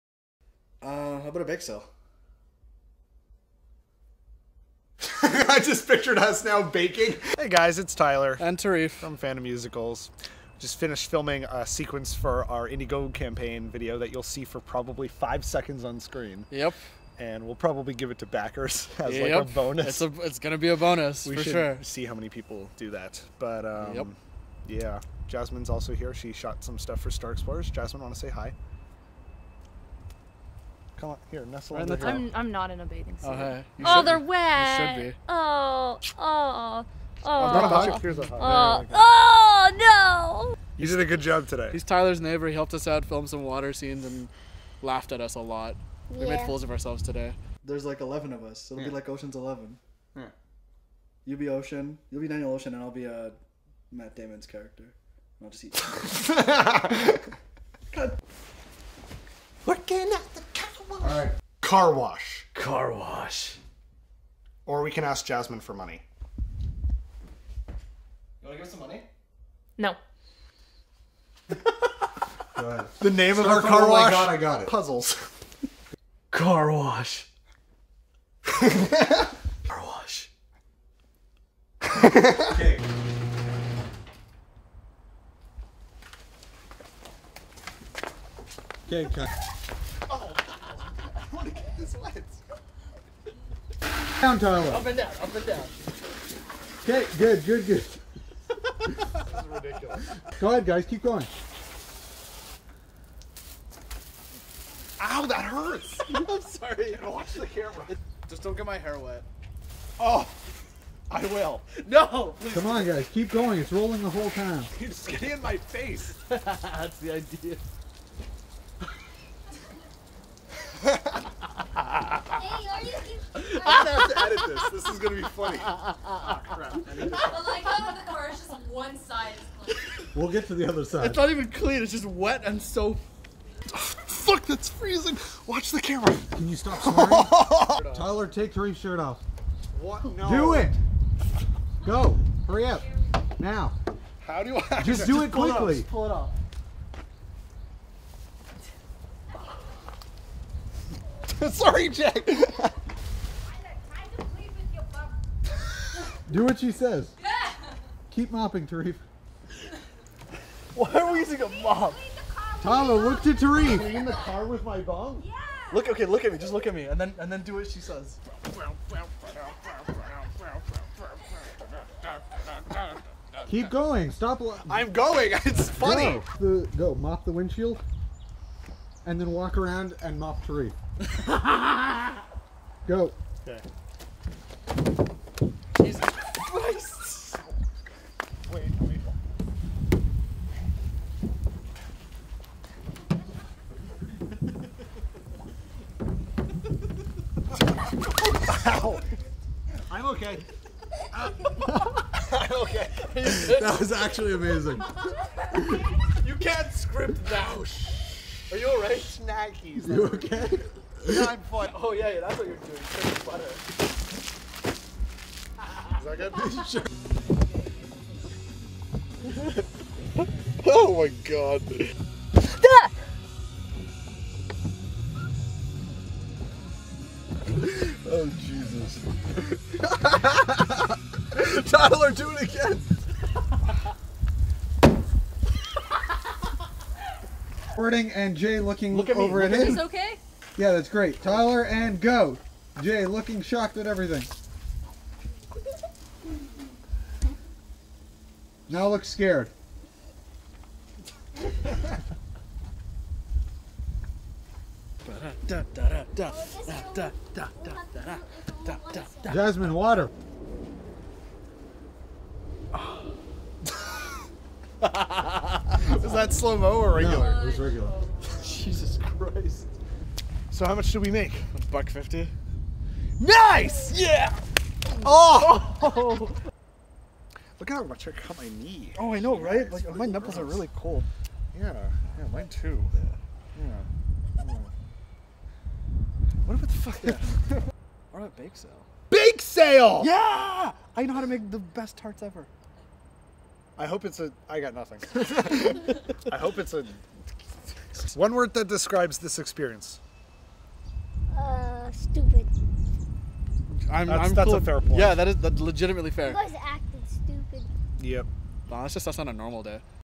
Indiegogo Car Wash Bloopers
The making of our Indiegogo Car Wash video was all kinds of uncomfortable and hilarious, with lots of random takes and fun times. Take a look!